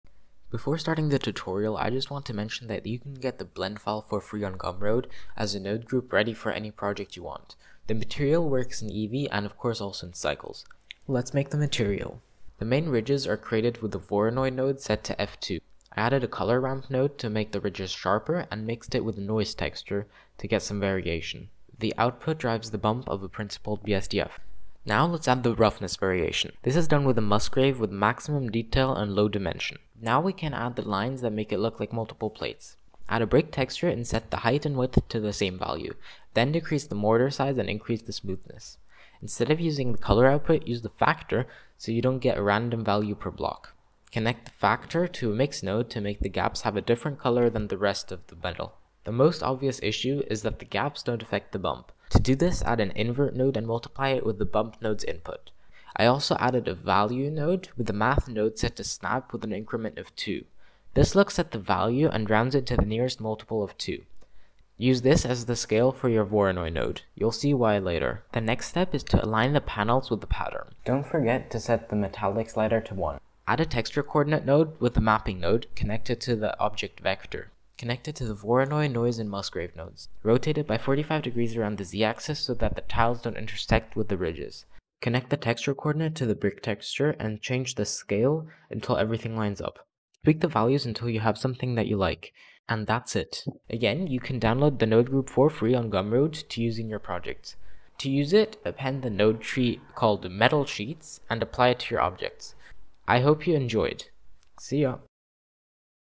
Procedural Metal Sheets in Blender
In this tutorial, you will learn how to make a procedural metal floor material in Blender.